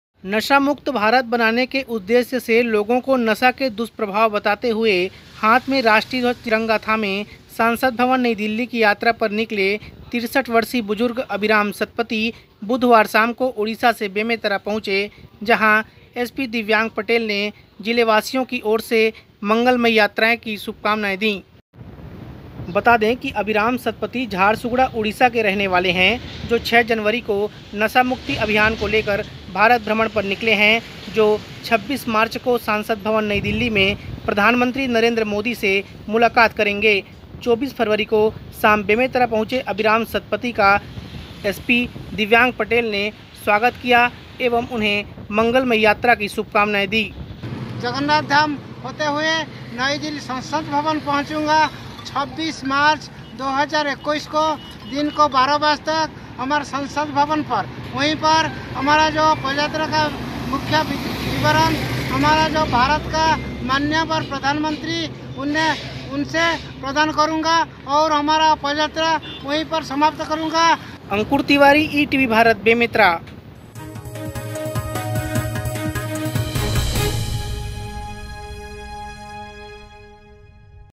From Delhi to Bemetara on foot! | ETV Bharat Chhattisgarh
नशा मुक्त भारत का सपना लेकर ओडिशा के अभिराम सतपथी दिल्ली तक पदयात्रा पर निकले हैं. अपनी यात्रा के दौरान अभिराम सतपथी बुधवार शाम बेमेतरा पहुंचे, जहां एसपी दिव्यांग पटेल ने उन्हें आगे की यात्रा के लिए शुभकामना दी.